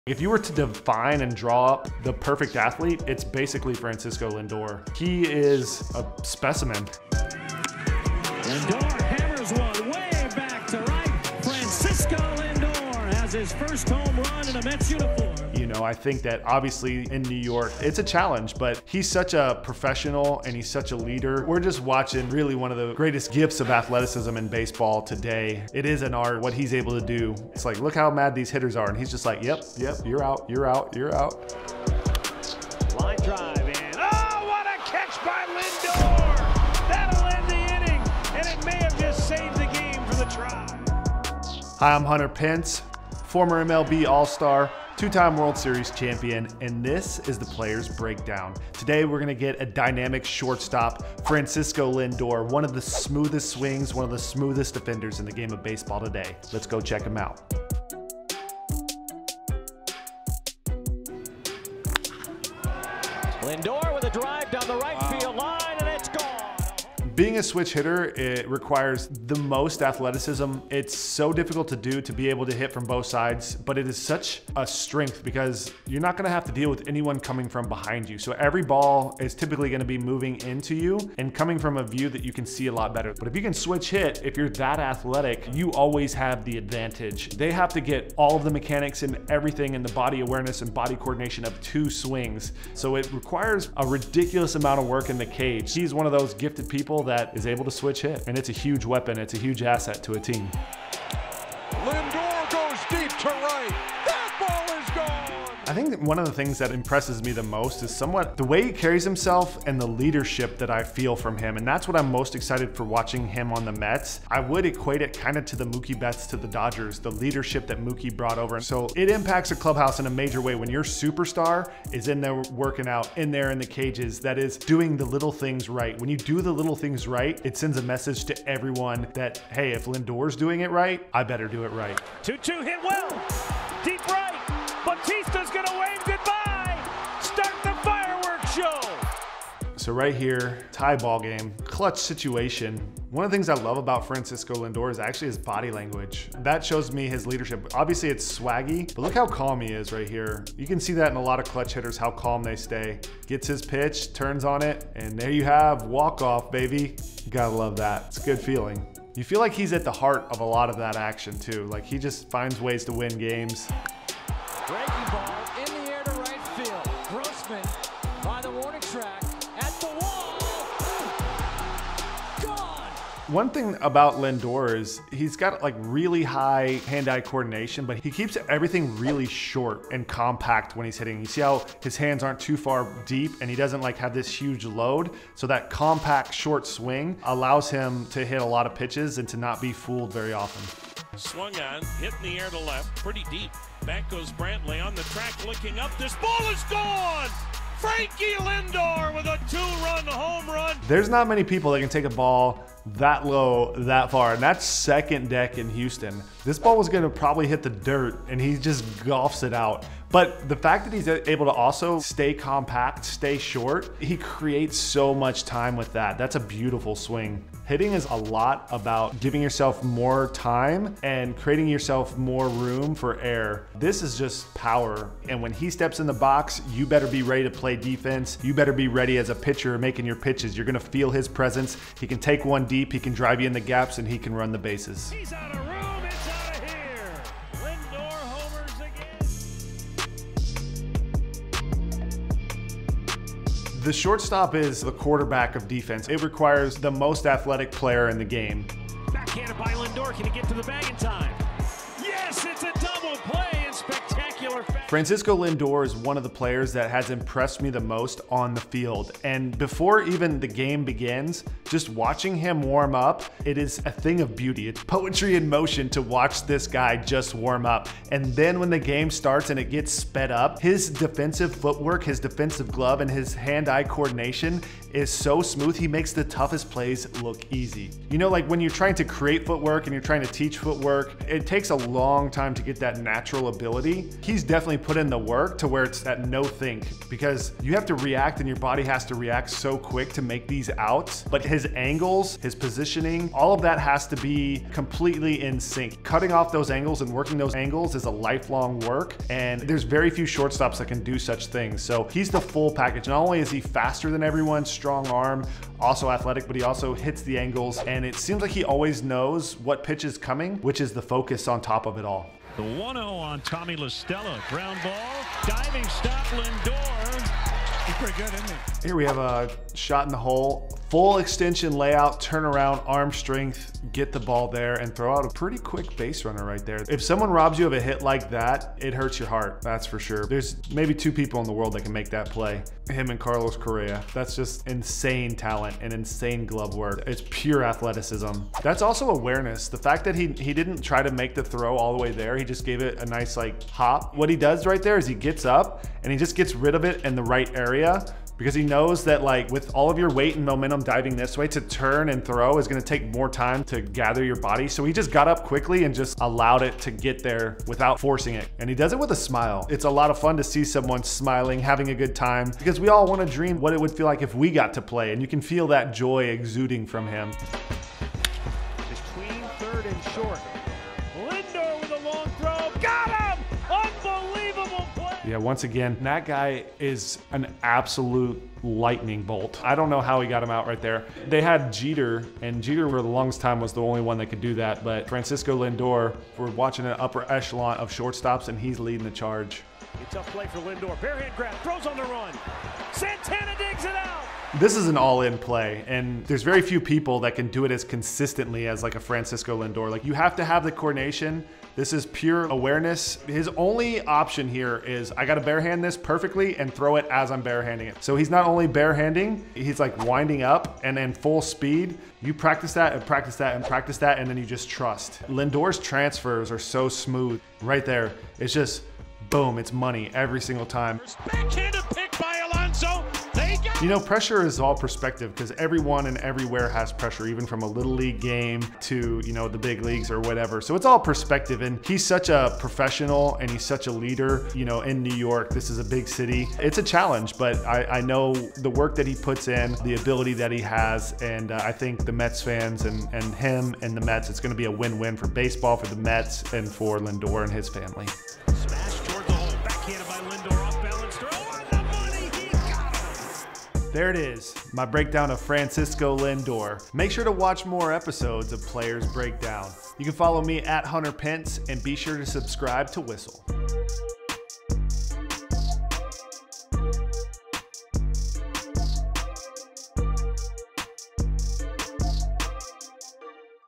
Why Francisco Lindor Is The BEST Shortstop In Baseball | Ft. Hunter Pence
COMMENT with a baseball player you want to see Hunter Pence break down on the next episode!